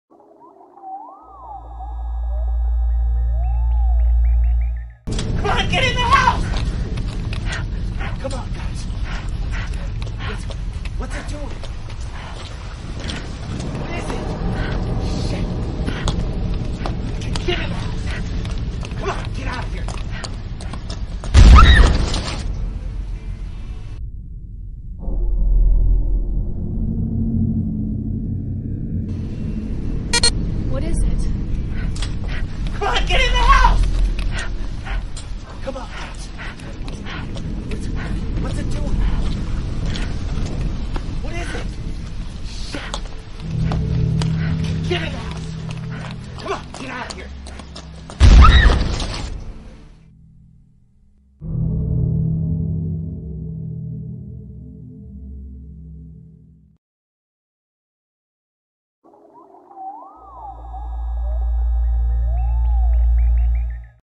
UFO Takes Off in front of Eyewitnesses footage from Brazil
→ 🛸 We Are Not Alone! We Never Were Alone! Discover the untold secrets of the universe through our captivating content.